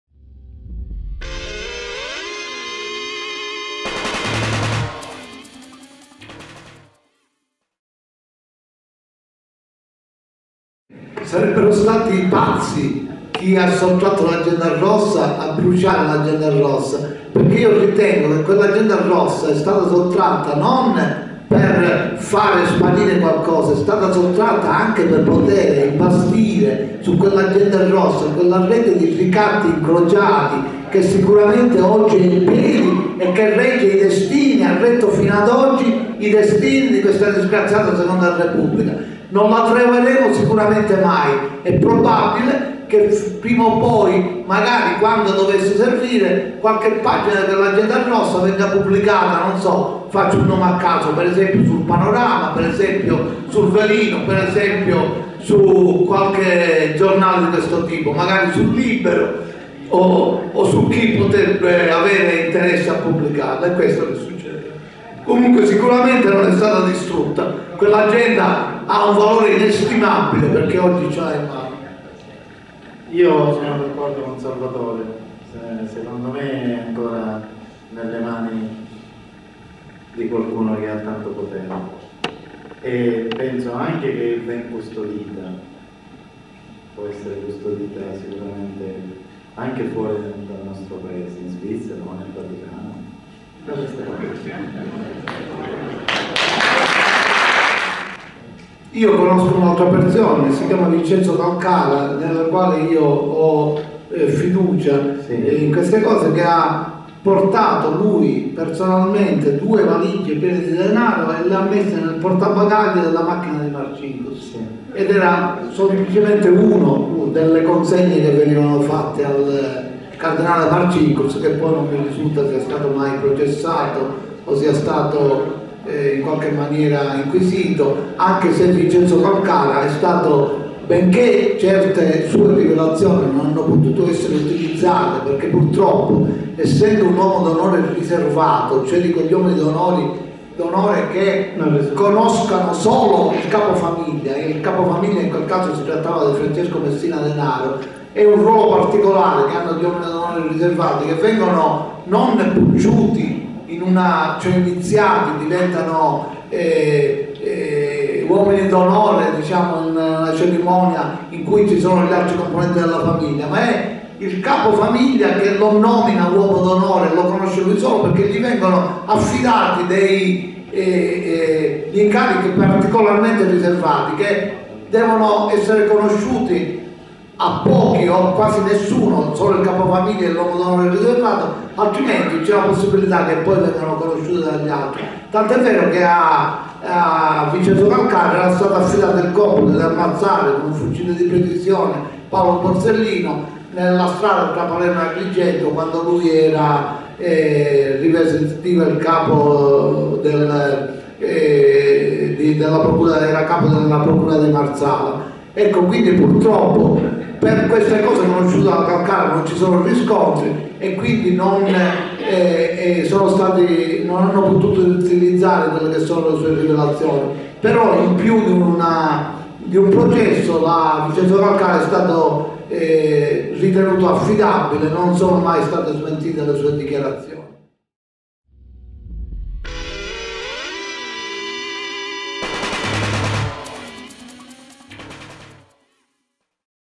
L'agenda rossa scomparsa e il vaticano (7/8)
Intervento del primo aprile 2011 di Salvatore Borsellino nell'auditorium della Regione a Pordenone.
Presentazione del libro "Gli ultimi giorni di Paolo Borsellino" di GIorgio Bongiovanni e Lorenzo Baldo presenti all'incontro.
Montaggio e riprese di Davide Franchini